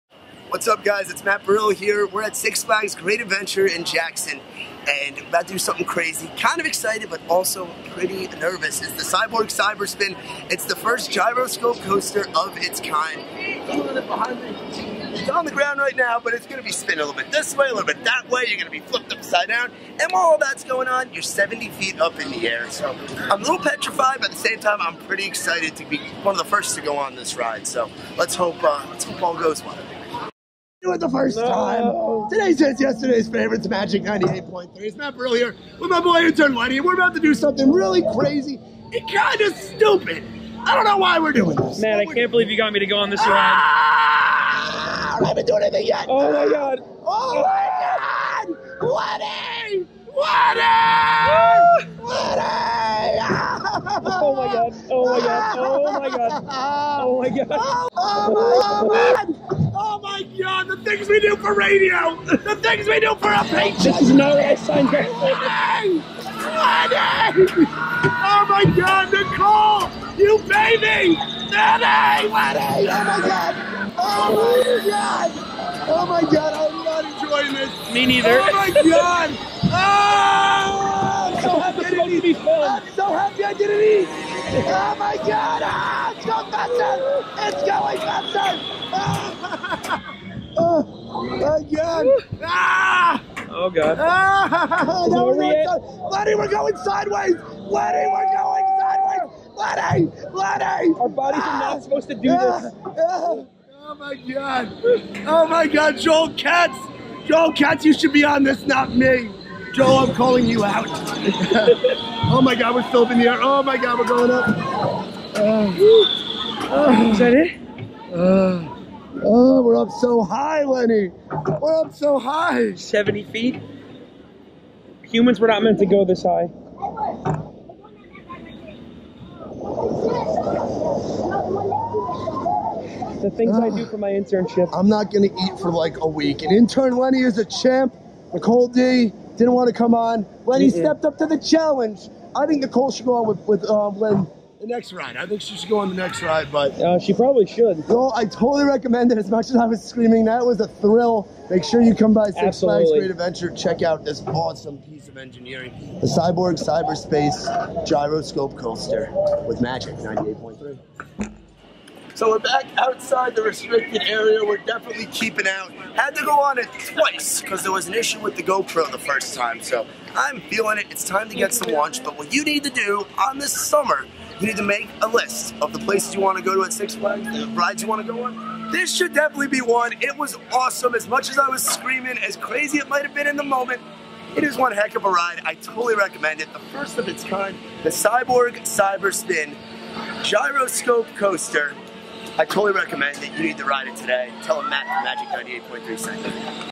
Matt Burrill tests the Cyborg Cyber Spin at Six Flags Great Adventure
Watch Matt Burrill and Intern Lenny tackle Six Flags Great Adventure's latest attraction, The Cyborg Cyber Spin!

Be sure to visit the ride at Six Flags Great Adventure in Jackson NJ